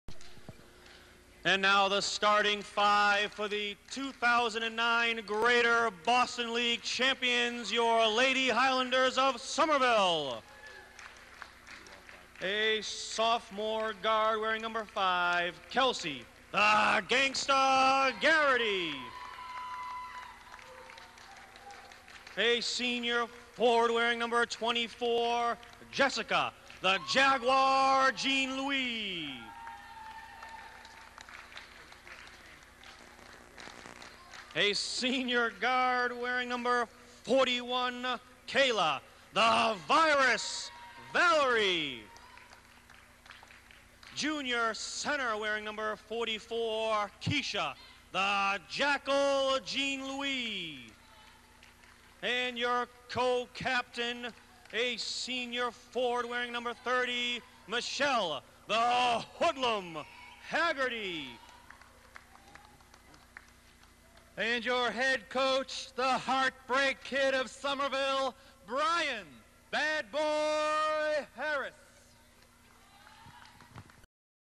Starting 5 v Everett 2 3 09
Bob introduces the Lady Highlanders starting five.